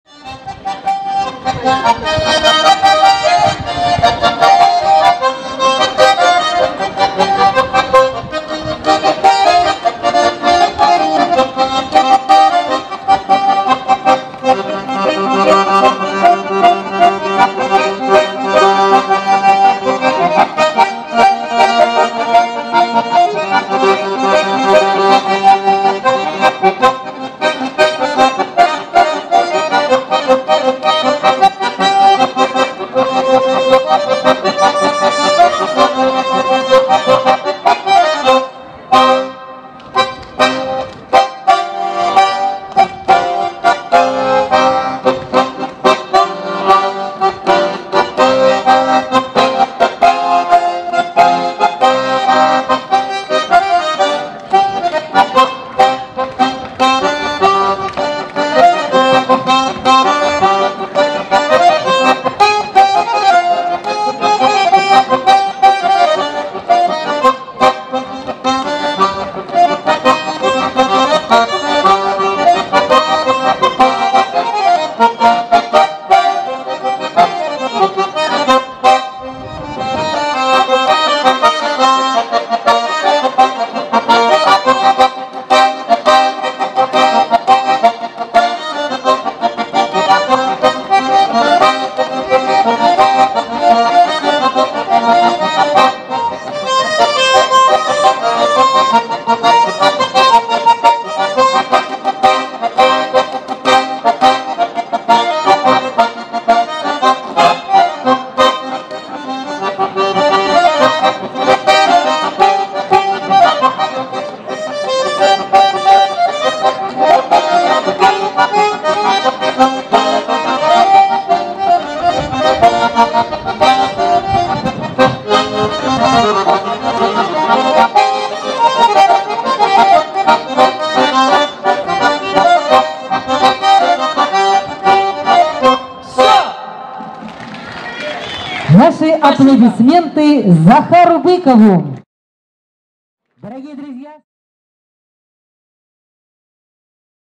Захар Быков, молодой рояльщик из Ельца
Захар Быков (Елец), елецкая рояльная гармонь. Попурри на темы народных песен. Снято 6 сентября 2020 года в Ельце на Праздничных Гуляниях  "Любимица народная - гармонь!" специально для проекта "Елецкая рояльная гармонь", А.Пчелинцев